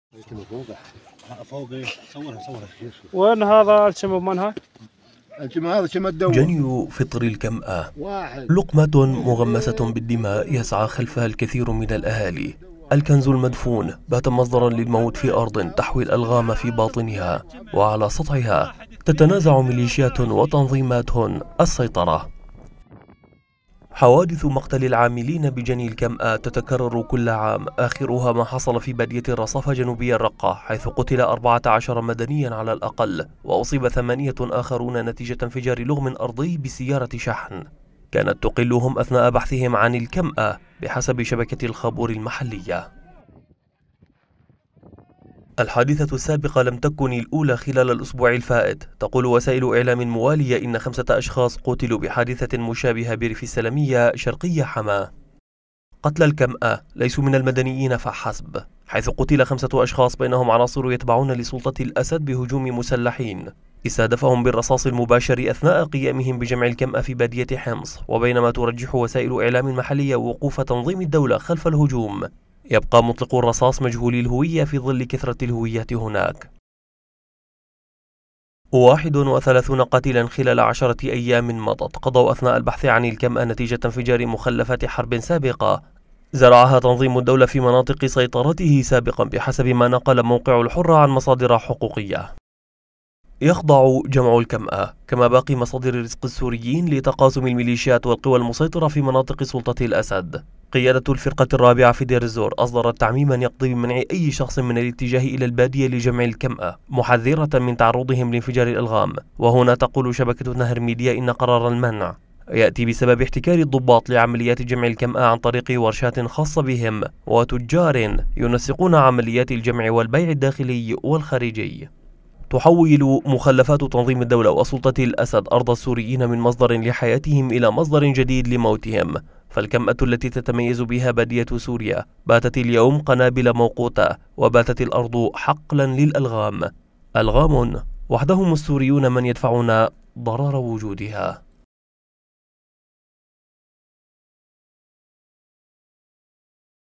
مخلفات الحرب في البادية تحصد عشرات الأرواح من جامعي الكمأة
تقرير: ريان الحمصي